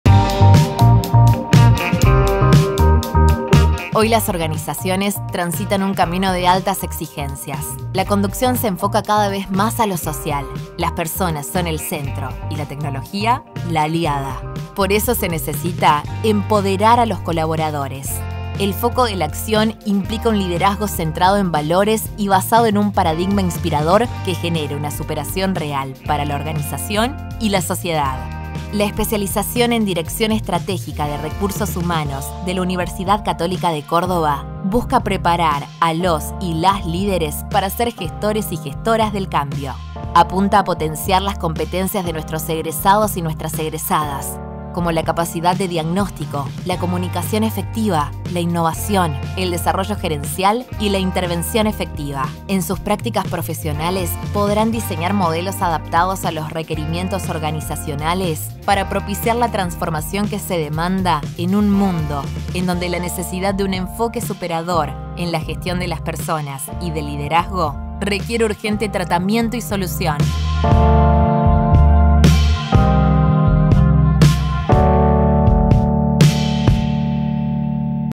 Especialización en Dirección Estratégica de Recursos Humanos
La Especialización en Dirección Estratégica en Recursos Humanos es una propuesta de formación de posgrado para quienes han decidido tomar el desafío de liderar la transformación que necesitan las organizaciones, haciendo foco en las personas.